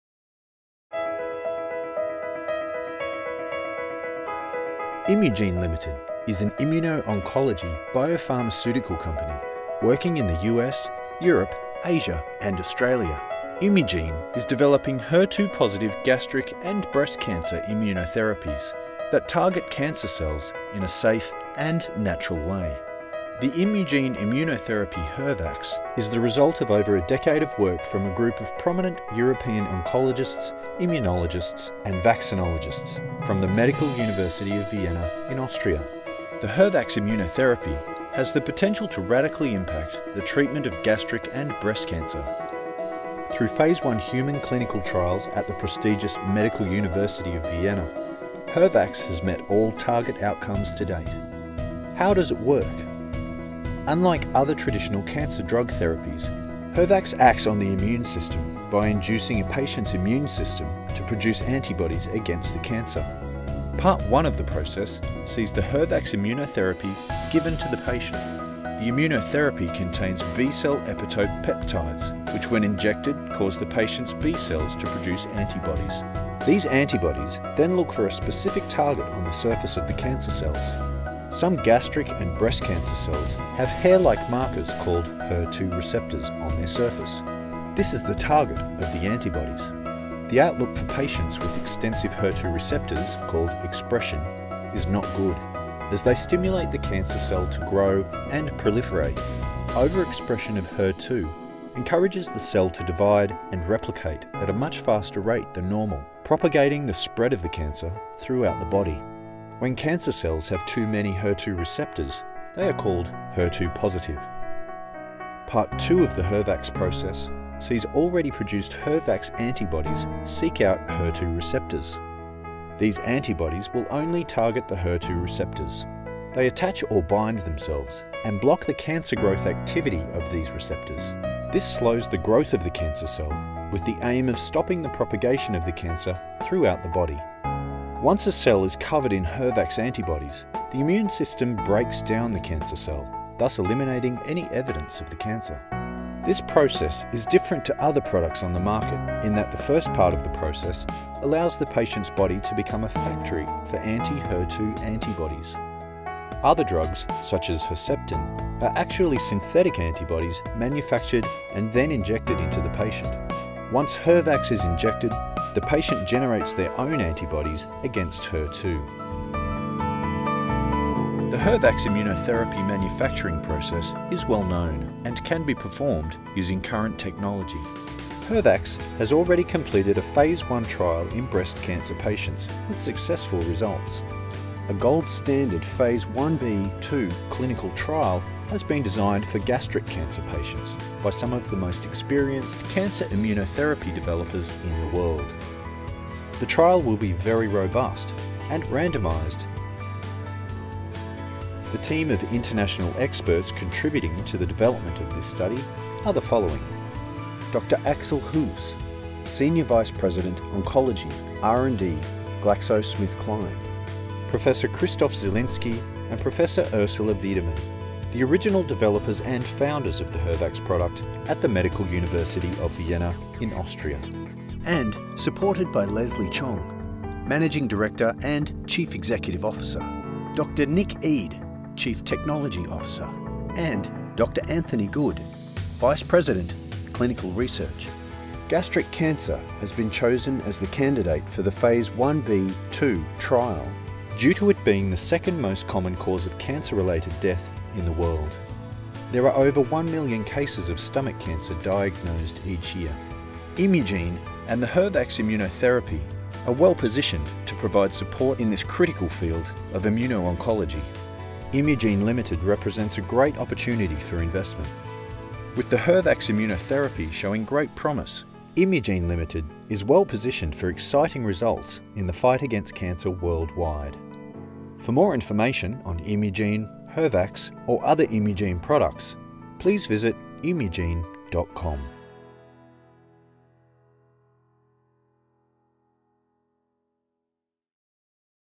IMU P01 CorpVid FINAL HD 1920x1080 2018 003
Imugene HER-Vaxx cancer vaccine video.  How it works.